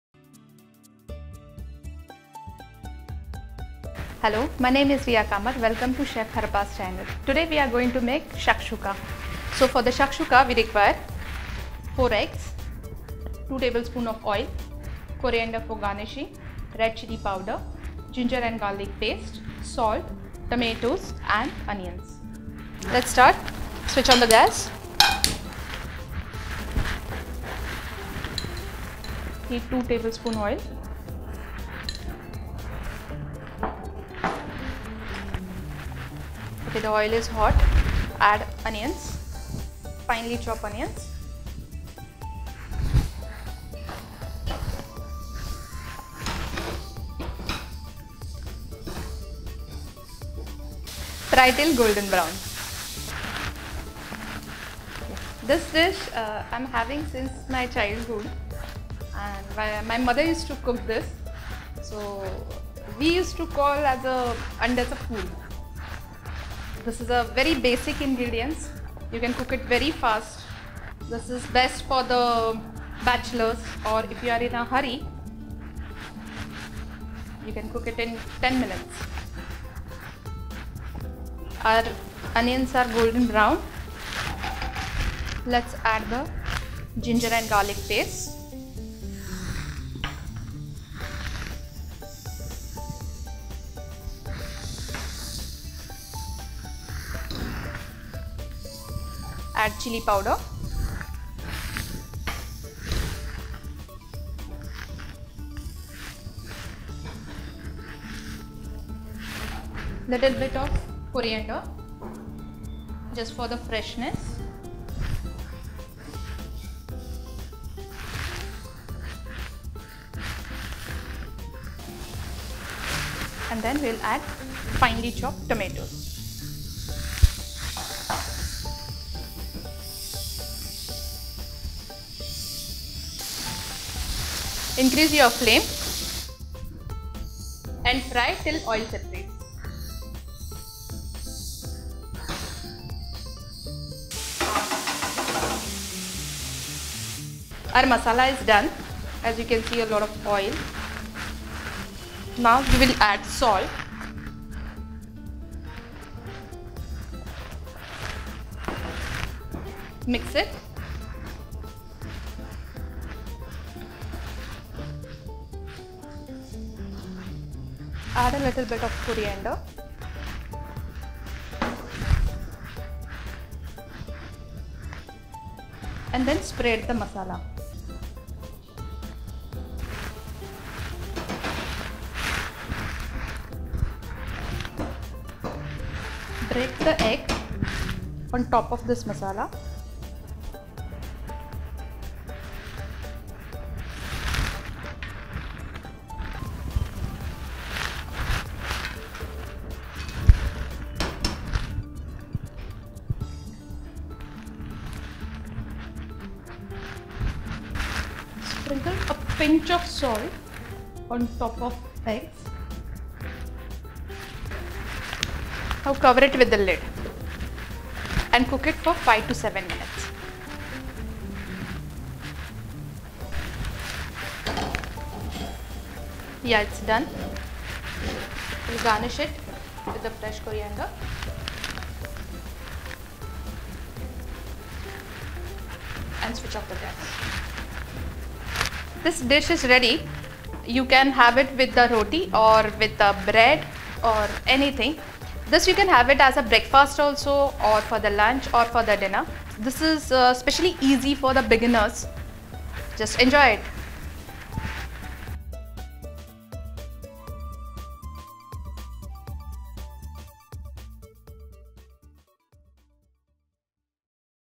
Shakshuka Recipe for Bachelor's | Egg Recipe I Chef Riya Kamat
The Perfect Egg recipe Mangalorean style for bachelors as shown  by our home Chef Riya Kamat

SHAKSHUKA

chopped onions - 3 medium sized
Chopped tomatoes – 1 cup

Method-

➢ Heat oil , add finely chopped onions & fry till golden brown.
➢ Add ginger & garlic paste fry for 2 minutes.
➢ Add chilly powder.
➢ Add finely chopped tomatoes & fry till oil separates.
➢ Crack the eggs over tomato mixture. Cover the pan & simmer for 10 minutes, or until the eggs are cooked.
➢ Garnish with chopped coriander. Serve hot with bread or roti.